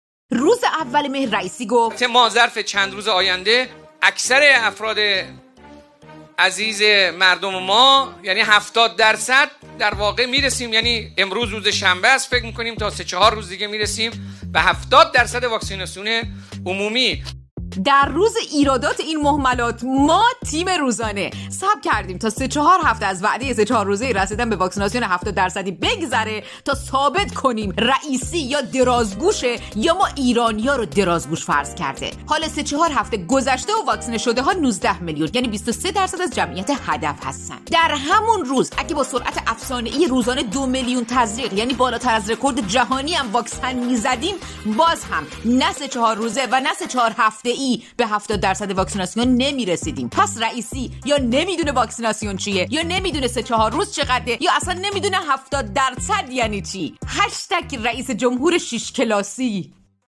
دروغ واکسیناسیونی رئیسی!
دوشنبه ۲۶ مهر

دروغ واکسیناسیونیی رئیسی

وعده‌های بی‌پایه و اساس تمامی نداره. اگر خودشون می‌دونند این وعده‌ها عملی نیست پس دروغ می‌گن! و اگر نمی‌دونند پس احمق هستند! به نظر شما دروغگو هستند یا احمق؟